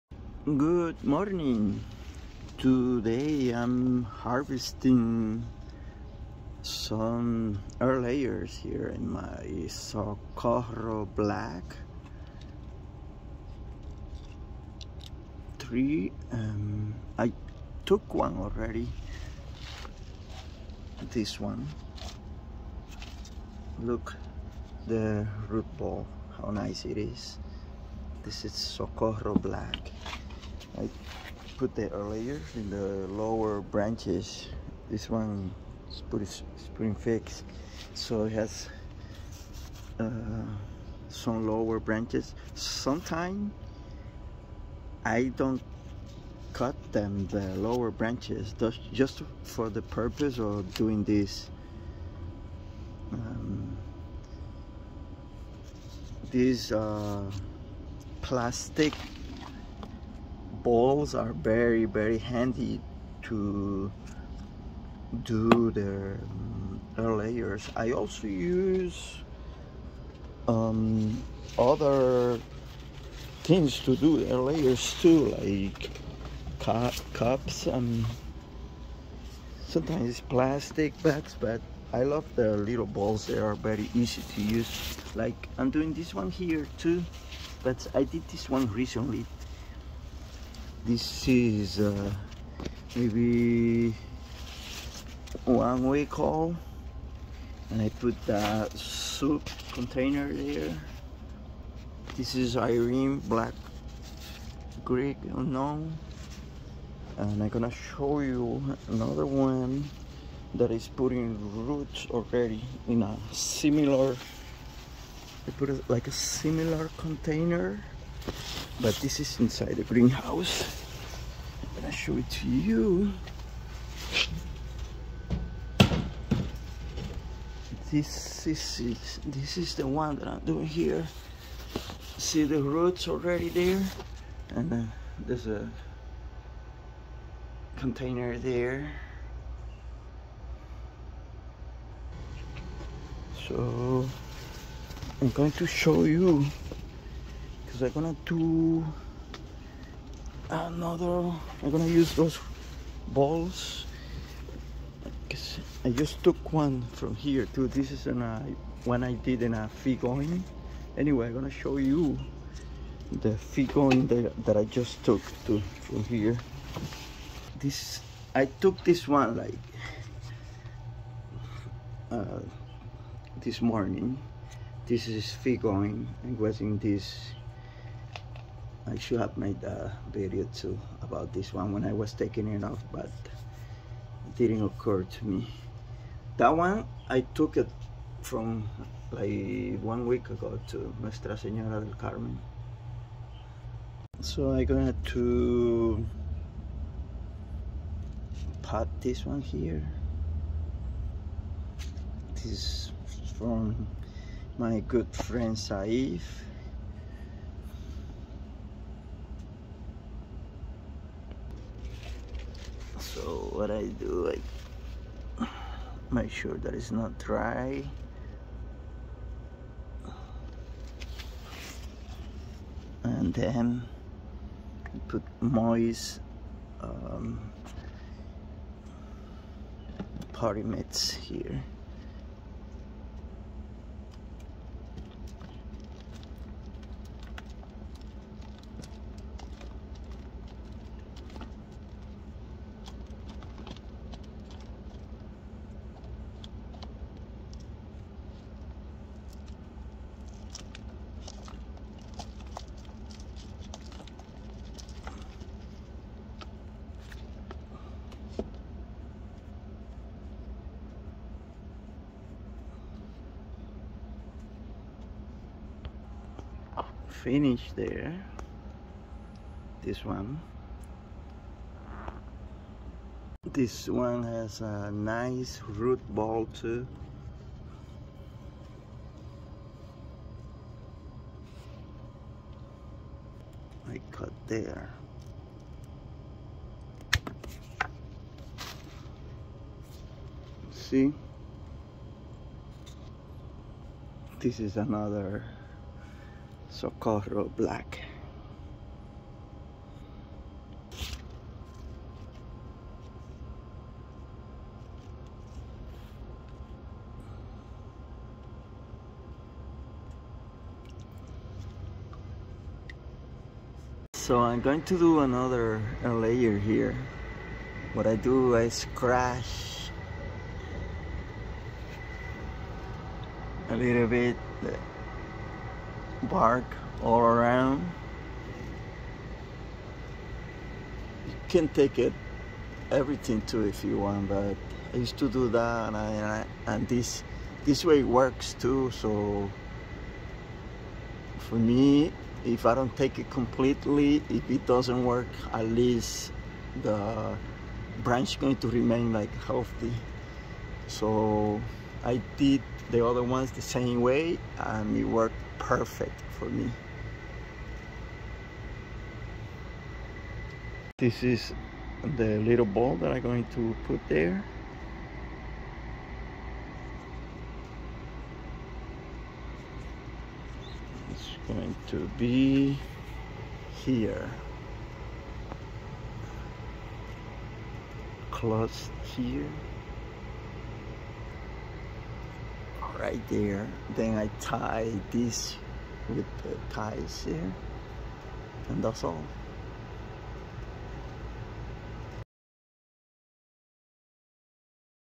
Harvesting and making airlayers#figs
Today, I harvested some airlayers that I did with rooting balls,I love to use these plastic rooting balls because they are easy to use, and you can reuse them over and over again 😉